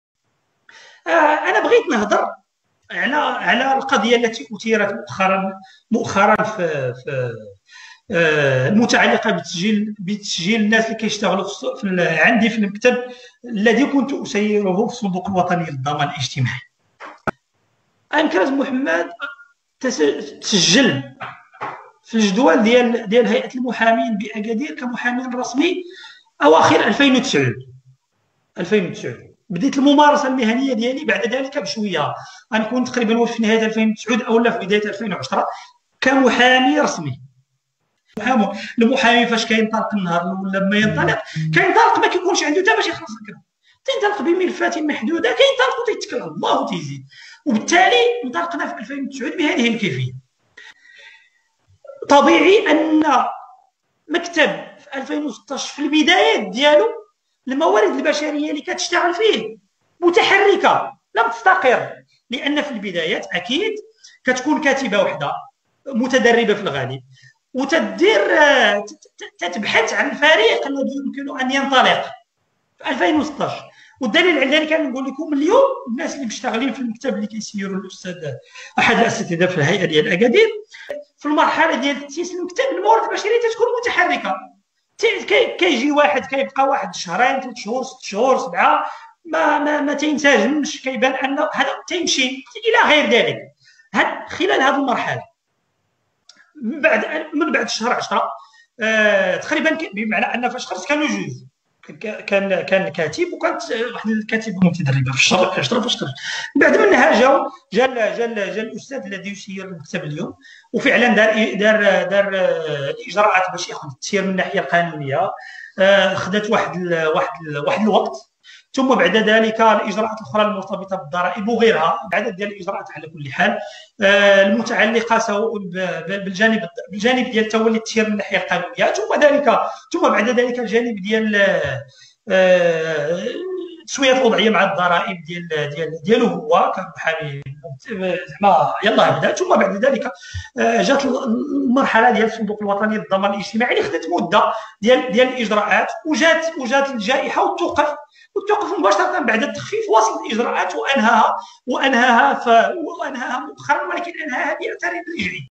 أمكراز يقدم تبريراته لعدم تسجيل مستخدمية في الضمان الإجتماعي
وزير التشغيل المغربي يفسر لعد تسجيل مشغلته بالضمان الإجتماعي